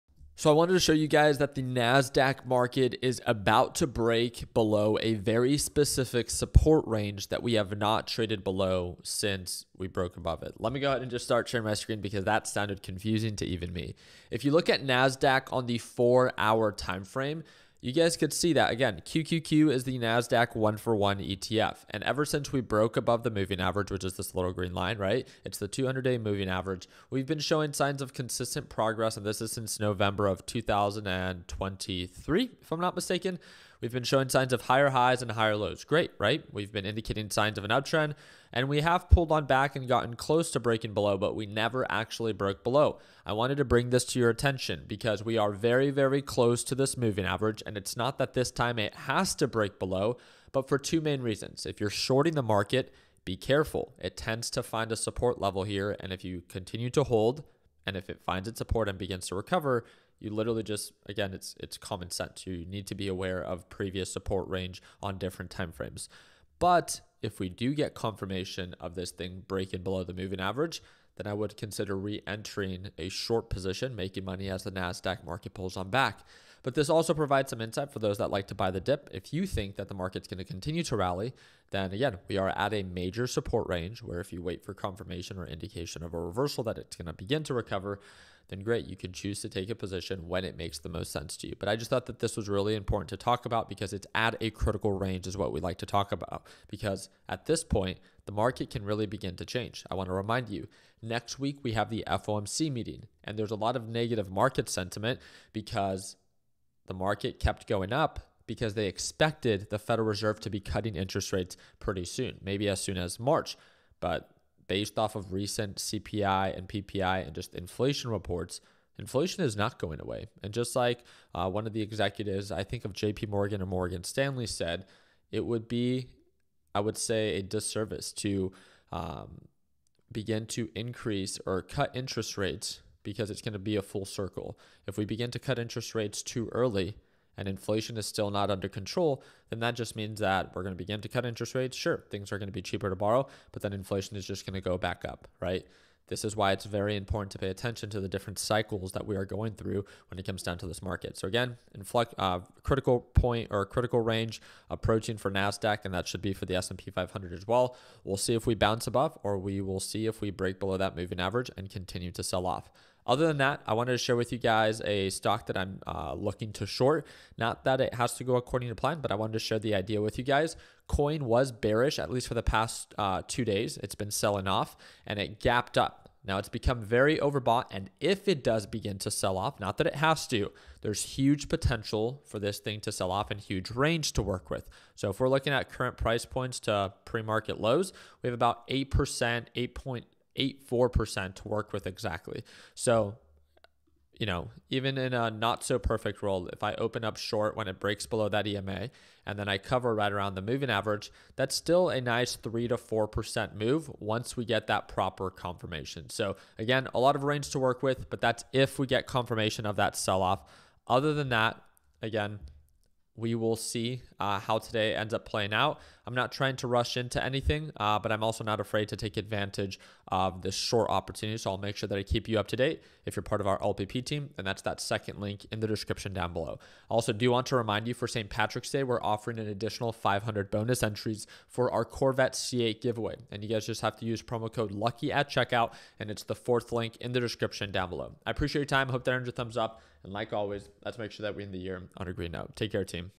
(WARNING) You Might Want To Sell Everything...
The Consumer Price Index (CPI) is a measure of the average change over time in the prices paid by urban consumers for a market basket of consumer goods and services. Indexes are available for the U.S. and various geographic areas. Average price data for select utility, automotive fuel, and food items are also available.

1. 🚨 Message me any questions

Beyond Meat (BYND) reported a big fourth-quarter loss late Tuesday. Revenue fell less than expected, though the fake-meat maker guided low on sales. Still, BYND stock skyrocketed overnight as the company announced plans to cut operating costs and other "right-sizing" moves.